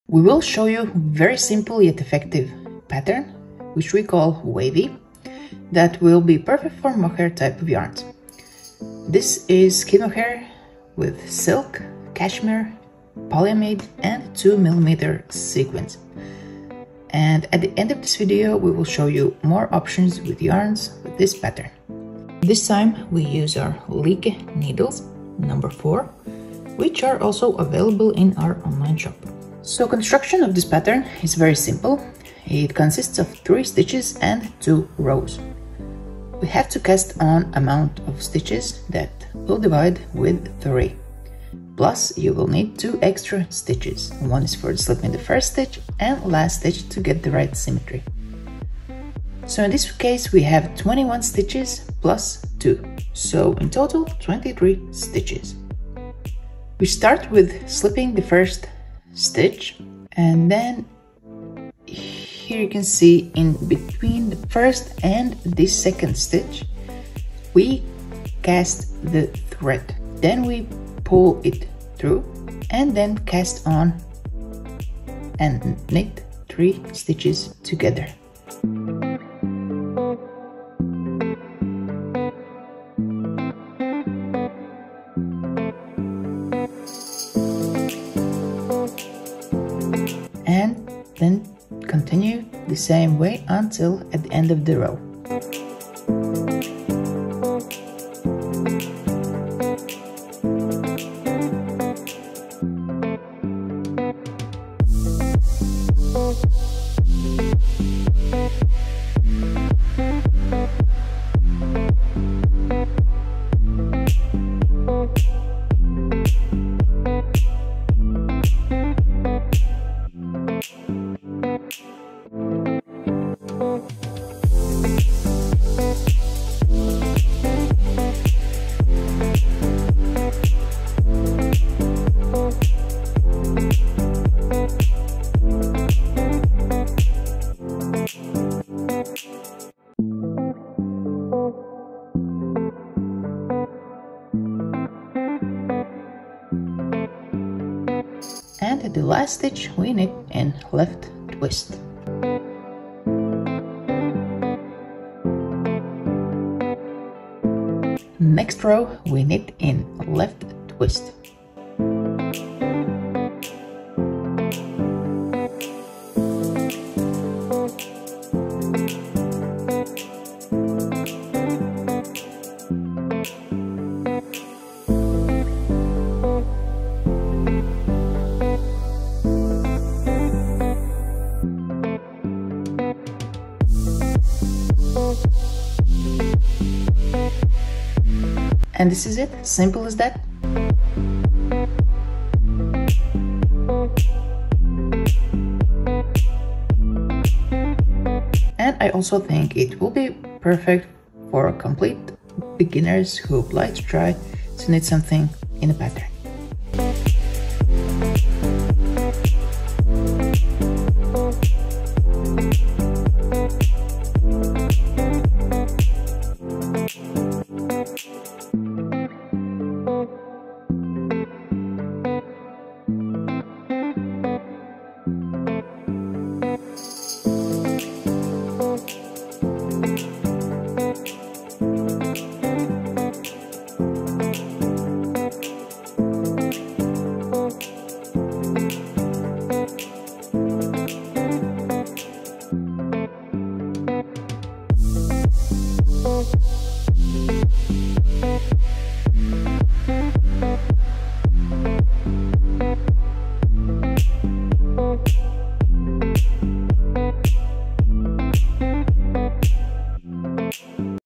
Knitting pattern "Wavy" by Wooly Yarn - perfect for beginners
Pattern "Wavy" - very simple yet effective pattern. Perfect for beginners and also experienced knitters!